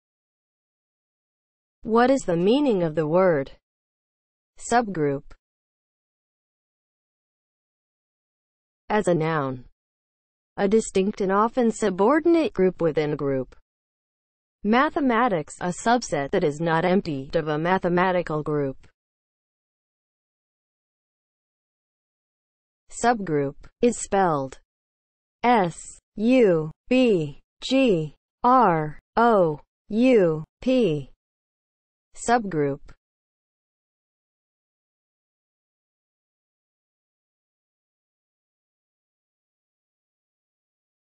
What is the meaning of the word SUBGROUP?
Definition and spelling of the word SUBGROUP.

(00:00) Intro
(00:07) As a noun
(00:21) Spelling

As a noun (00:07):
• a distinct and often subordinate group within a group
• (mathematics) a subset (that is not empty) of a mathematical group